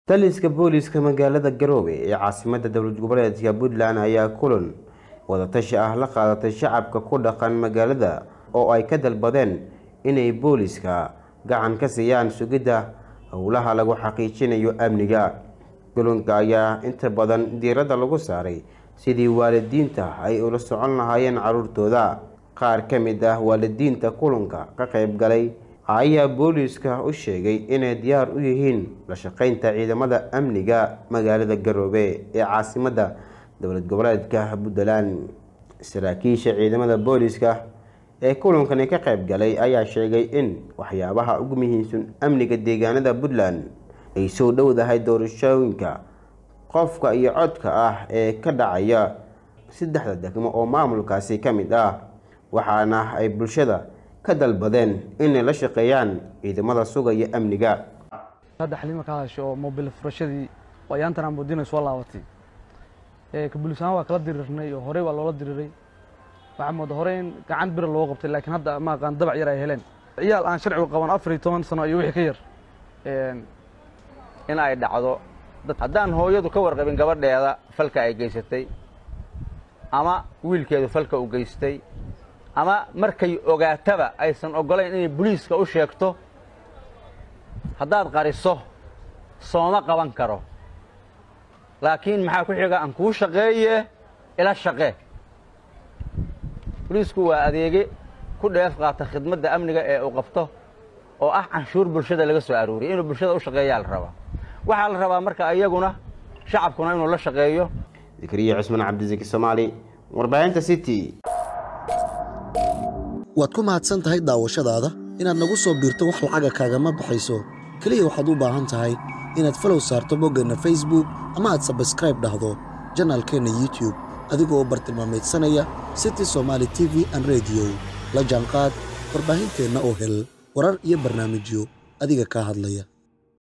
Booliiska Magaalada Garoowe oo Shacabka Magaalada ka dalbabay in la shaqeeyaan Ciidanka Amniga..
Booliiska Magaalada Garoowe oo Shacabka Magaalada ka dalbabay in la shaqeeyaan Ciidanka Amniga By Zeki Somali